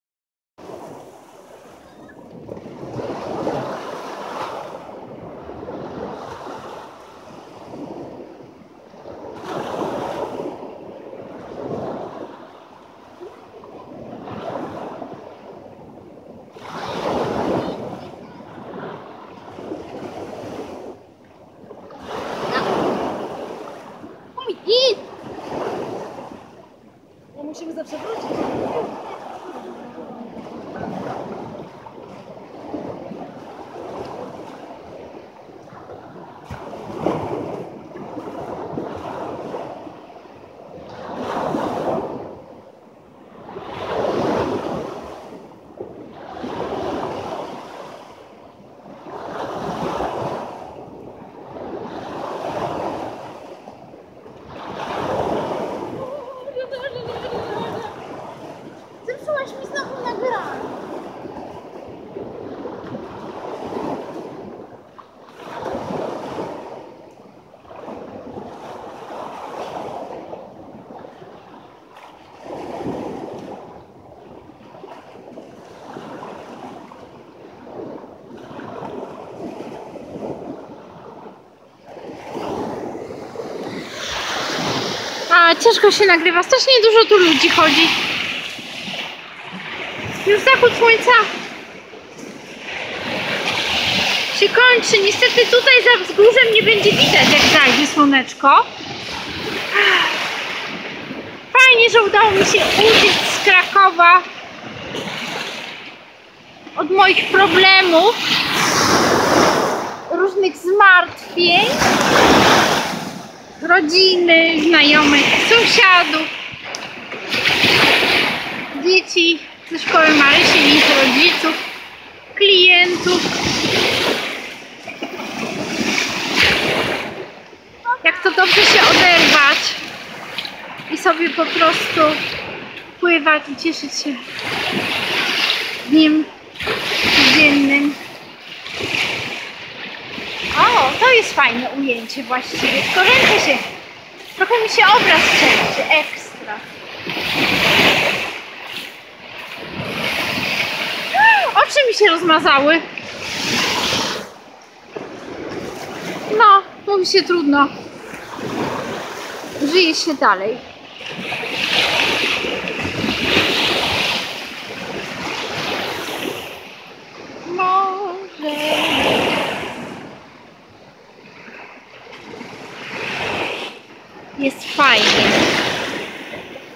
Kąpiel w morzu... Film z telefonu bez montażu (videodziennik)
A dzisiaj? Kolejny dzień nad morzem... Film z telefonu bez montażu (videodziennik). Jak to dobrze, że udało mi się znowu uciec od codziennych problemów...